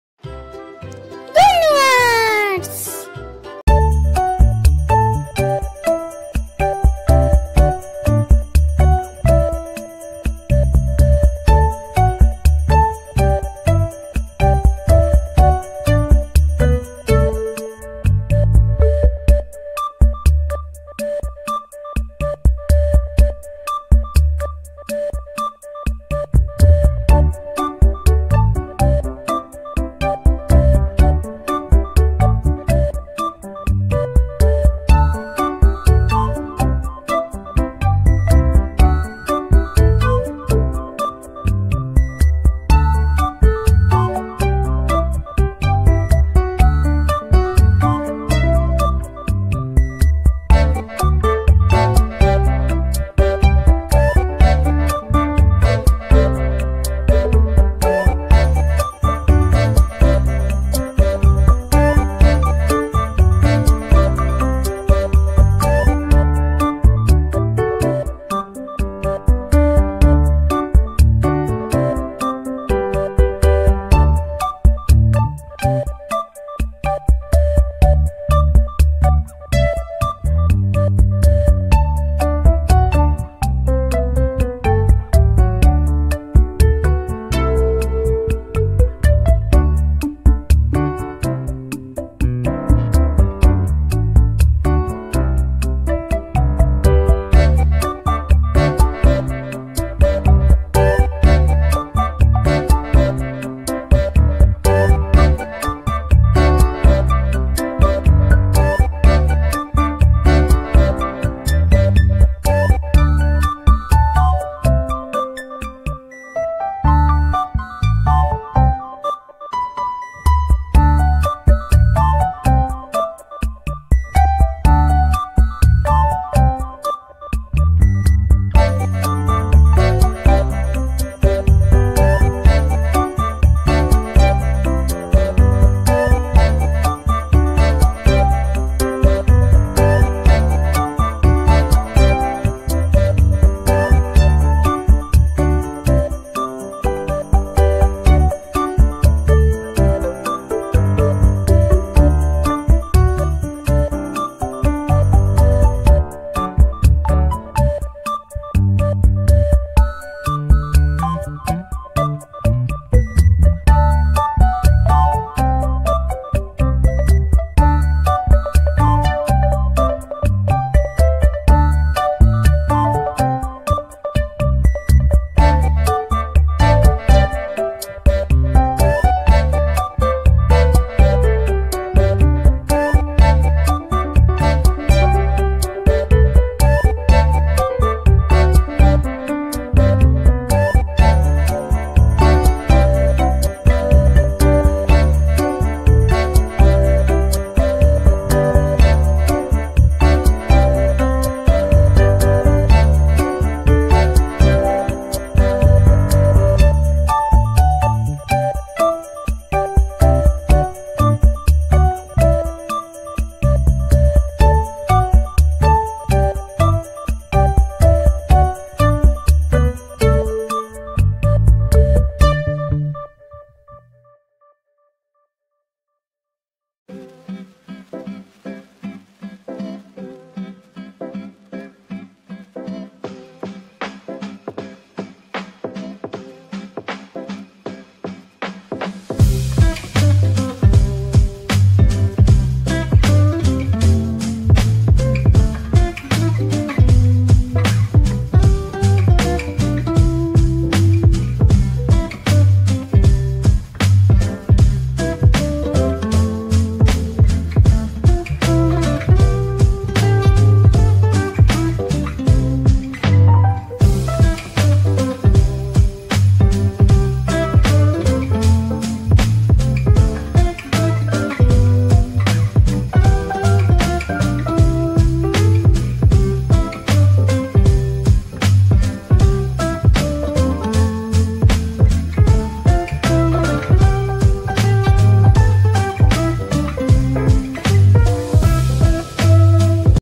How to draw makeup for kids || makeup drawing and colouring
Hello Friends🤩.
gunnu arts me aapka swagat hai.
Dosto aaj ki vedio me hum makeup kit draw aur colour krenge.